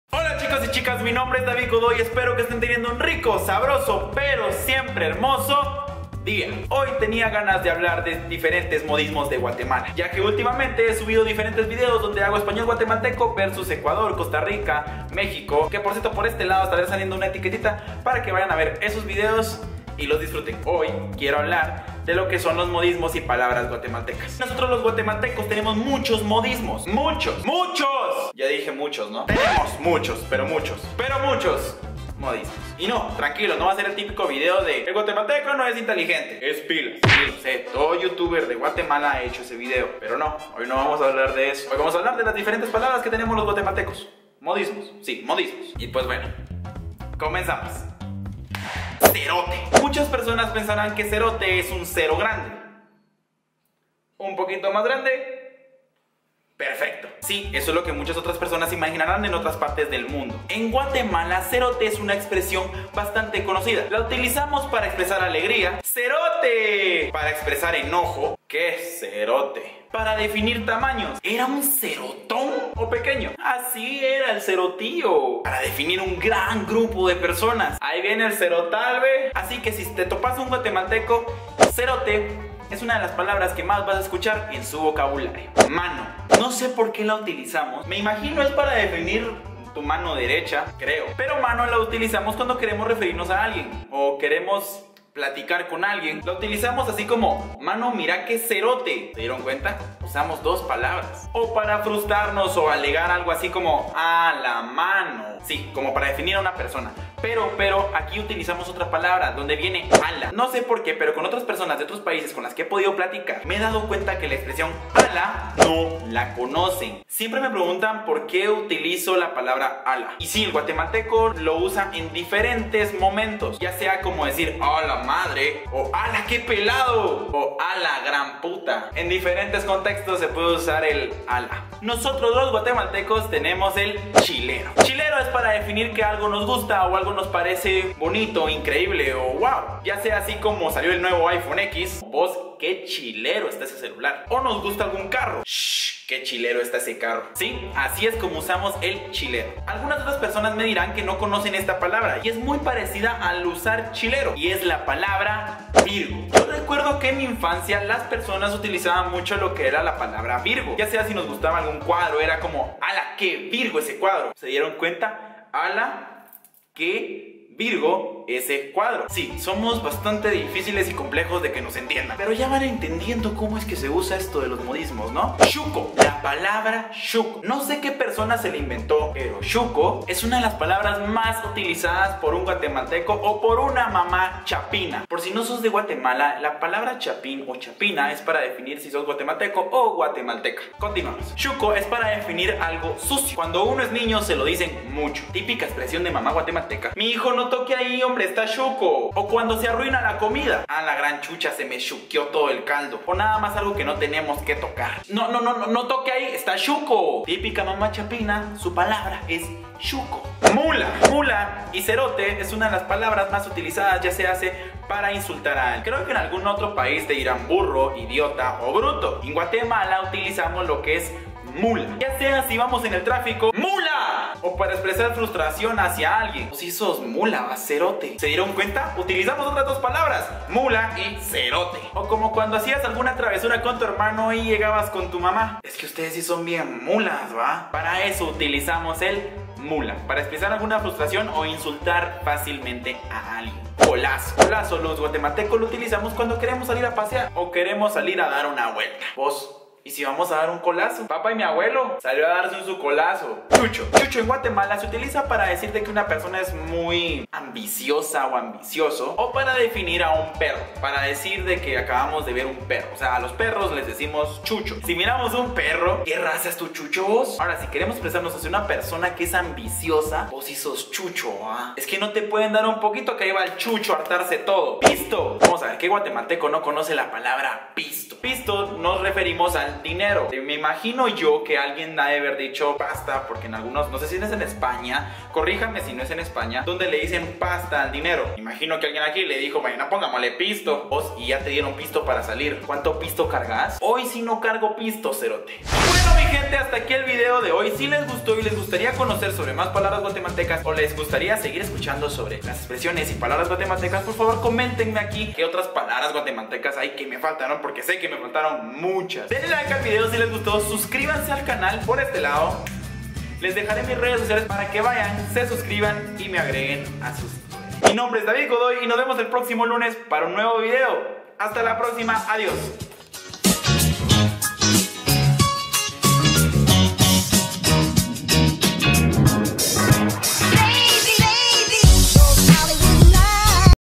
PALABRAS CHAPINAS | Modismos guatemaltecos | Guatemala Slang | Chapines | ESPAÑOL GUATEMALTECO
hoy quise hablarles y enseñarles unos cuantos modismos sobre mi país, espero que lo disfruten!!!